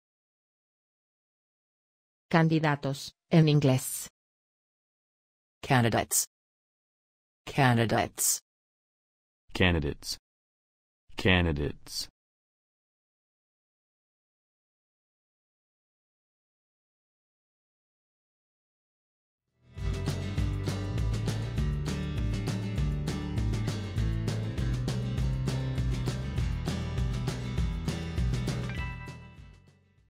Cómo se dice candidatos en inglés
¿Cómo se dice correctamente candidatos en inglés? Escuche con nosotros.
¿Cuál es la traducción correcta de la palabra candidatos del español al inglés? Habla como los americanos. Además, ¿cómo escribir correctamente la palabra candidates?

¡Gracias!